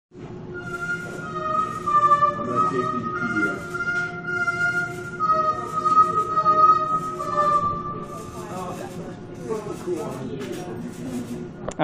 Laser Harp with fog machine effects
The MSP430 based laser harp, now with fog machine used to show beams.